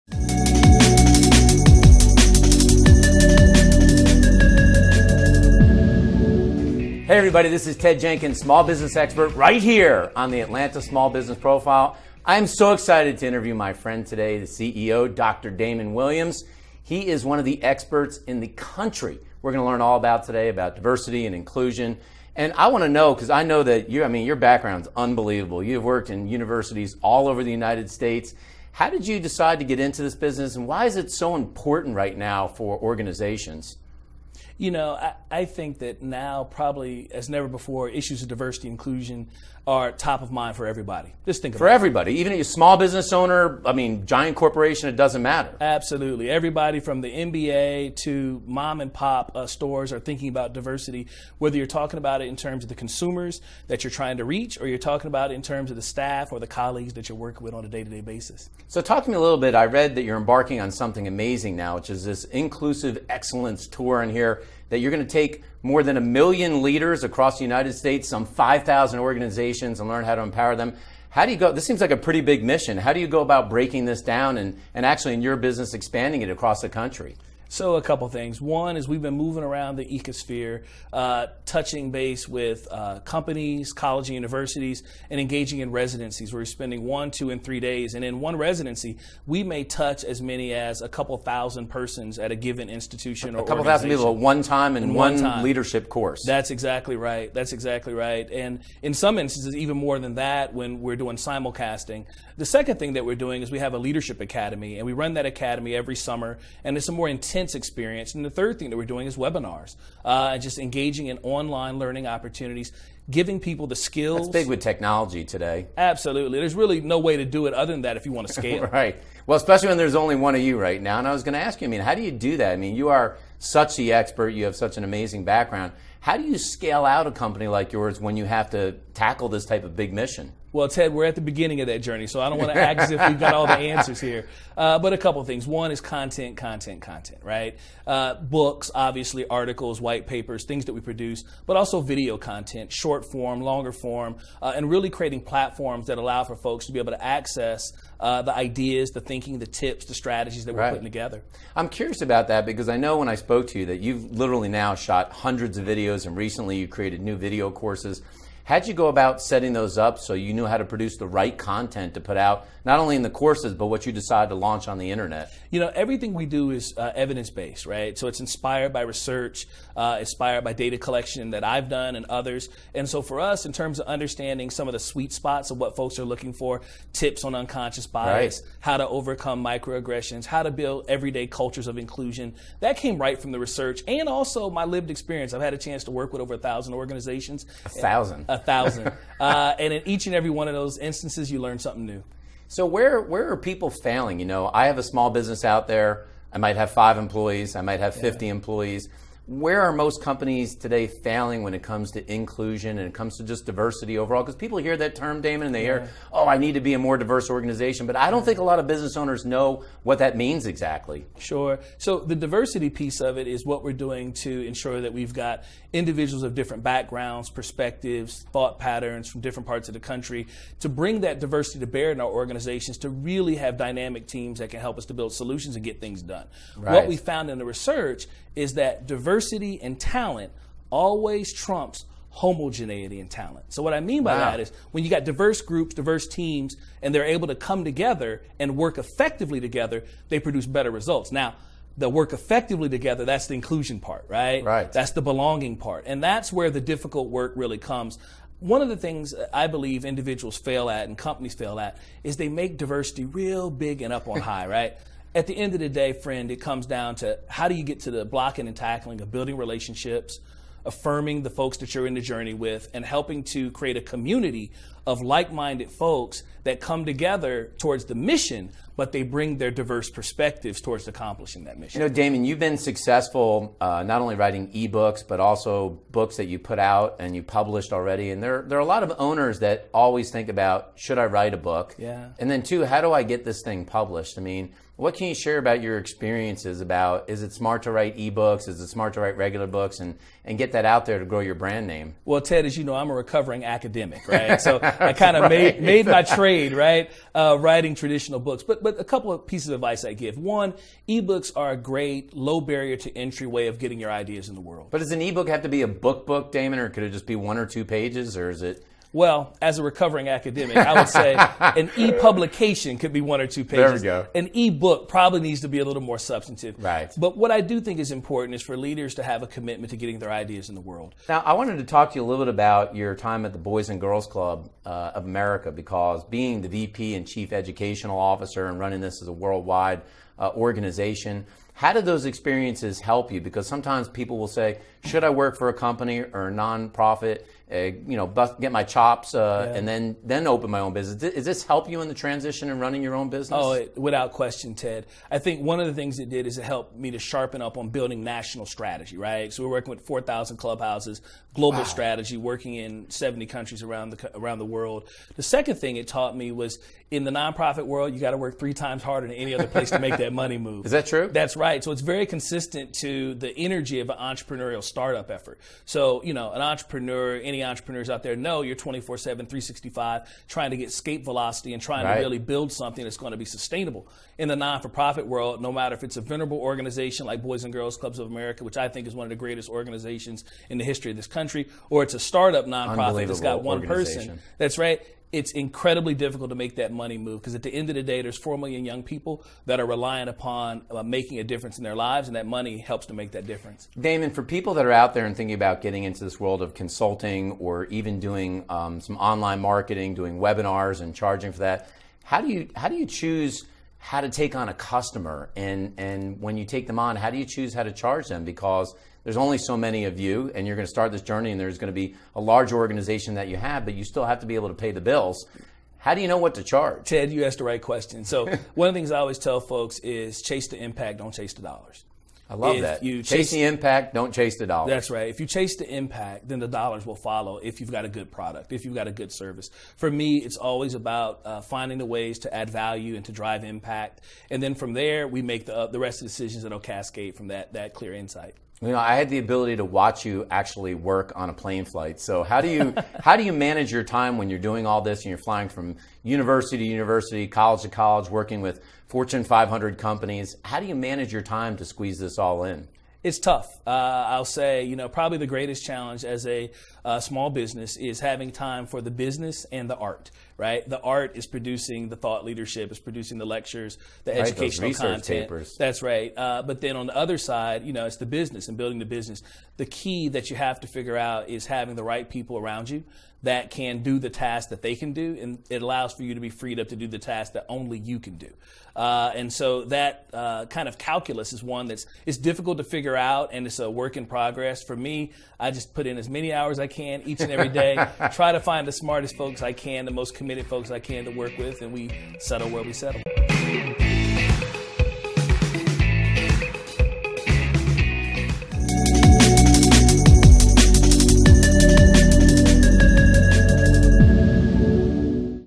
Dr. Damon A. Williams Talks About the Imperatives of Diversity & Inclusion
Learn more about Dr. Williams and his work empowering leaders to do more in the area of diversity, equity, and inclusion.  Dr. Williams sits down and talks with Mr. Ted Jenkin and the Atlanta Small Business leadership forum.